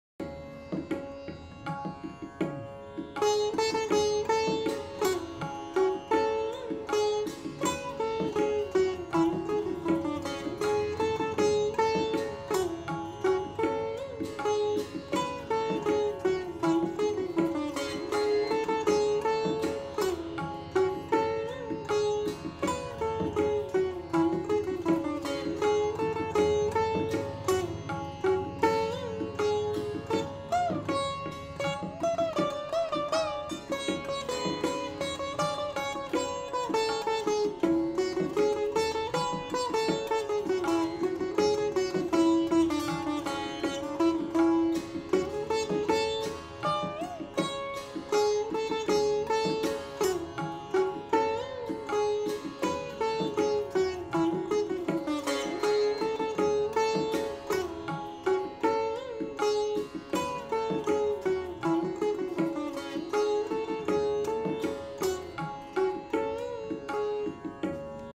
Bhairavi Jhaptal 10 Matra Gat. झपताल भैरवी गत
भैरवी गत झपताल में.
Jhaptal is set to slow tempo; therefore you call it bada khyal gat as well (बड़ा ख़्याल).